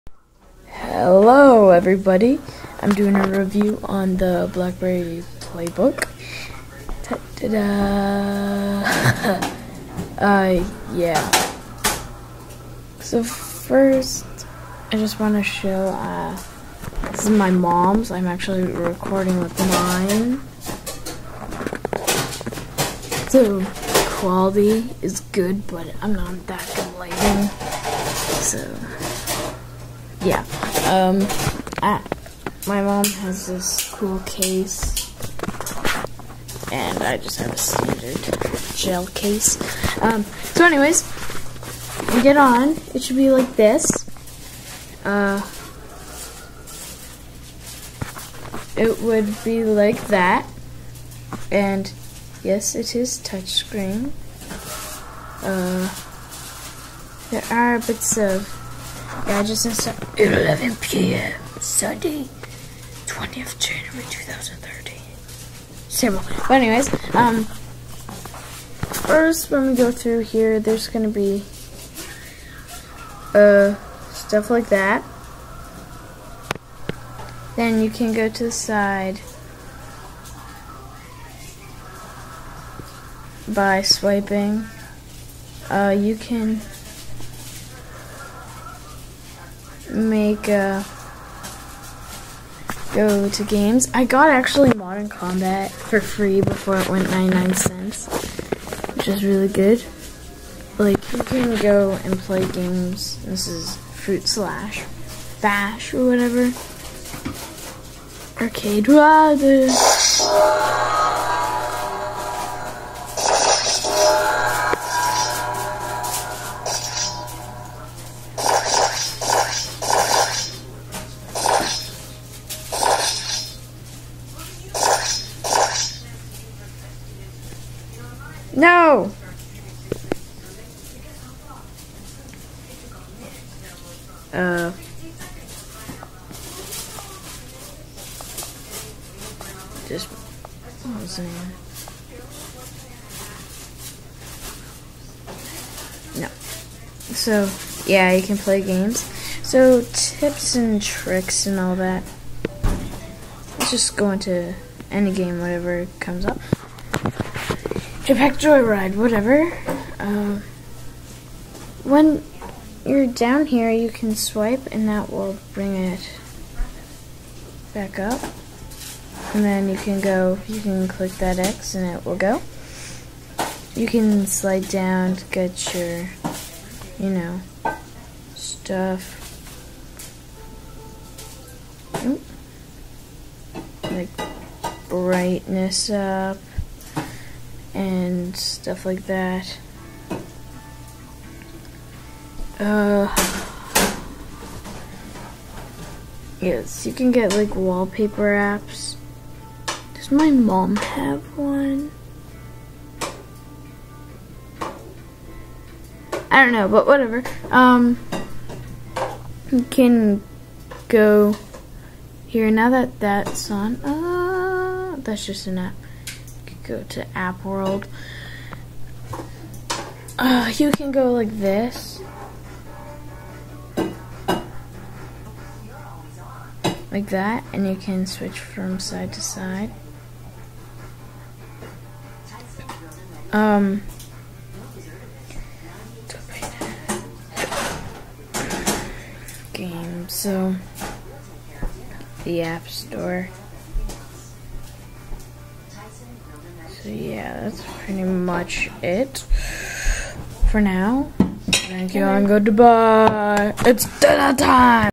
playbook review
I have my own playbook and I used my moms